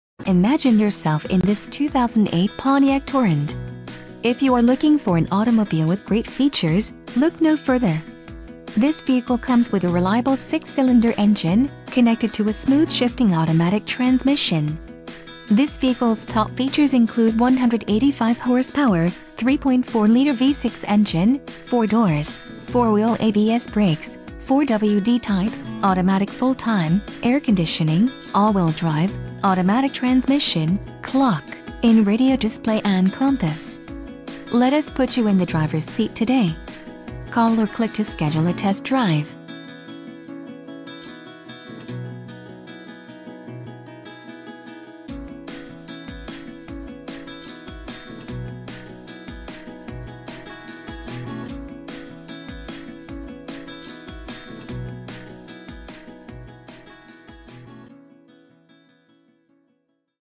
2008 Pontiac Torrent - Benny Boyd Marble Falls - Marble ...
This 2008 Pontiac Torrent  Has A Clear Includes a CARFAX buyback guarantee.. . This Torrent Is Equipped With Power Locks and Windows. This Torrent Is Priced to Sell Please Contact our Internet Department For the SPECIAL Internet Price and a Haggle Free Shopping experience.